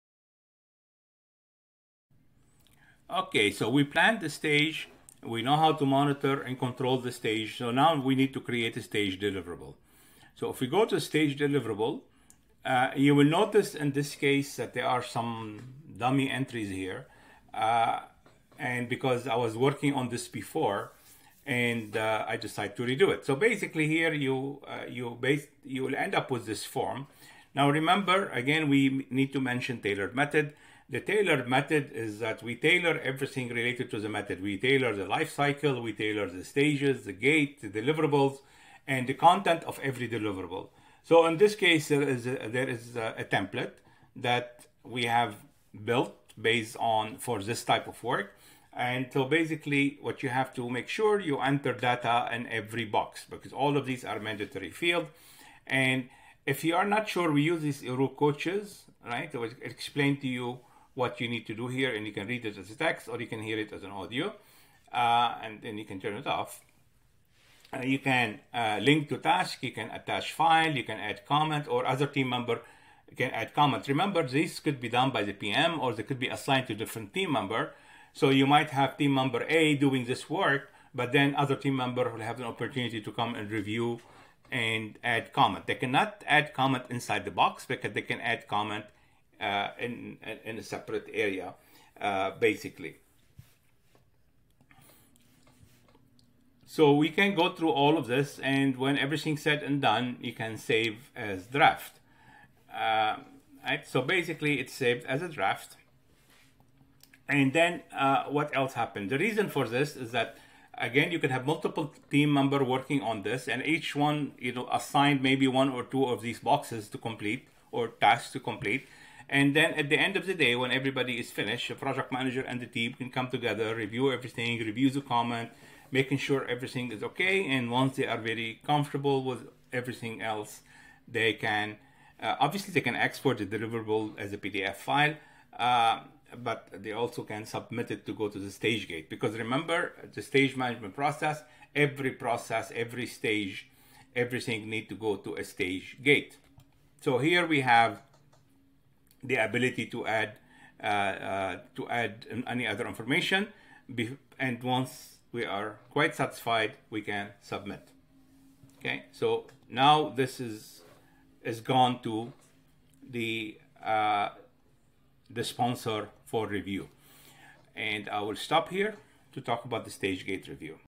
PLC Management 3, Develop a Stage Deliverable
In this video, we're focusing on creating a stage deliverable in Project Life Cycle (PLC) management.